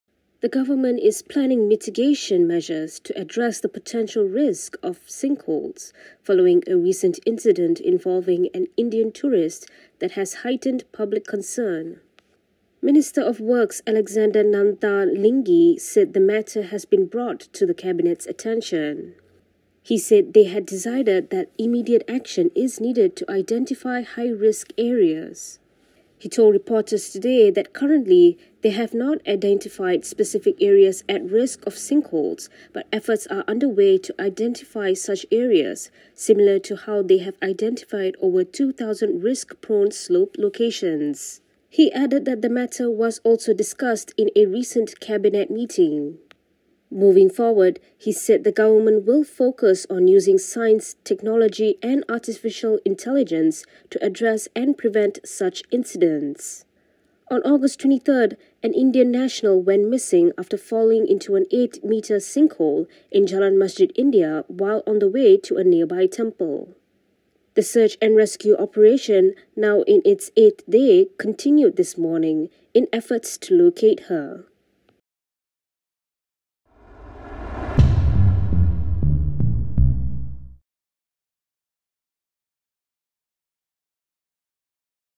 Minister: Govt planning mitigation measures to address sinkhole risks
The government is planning mitigation measures to address the potential risk of sinkholes, following a recent incident involving an Indian tourist that has heightened public concern.

Minister of Works Alexander Nanta Linggi said the matter has been brought to the Cabinet's attention, which has decided that immediate action is needed to identify high-risk areas.